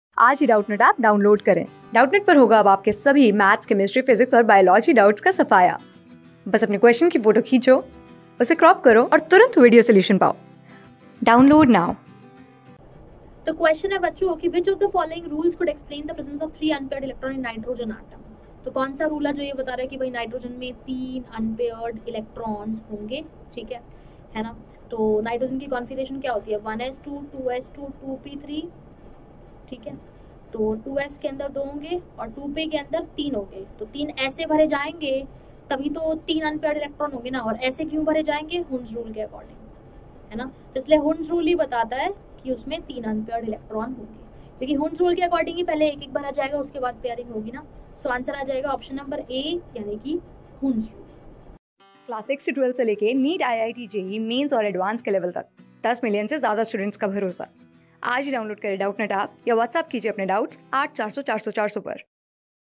Which of the following rules could explain the presence of three unpaired electrons in N-atom?
Class: 11
Subject: CHEMISTRY
Chapter: Atomic Structure
Board:INTERNAL

👉 Have Any Query? Ask Us.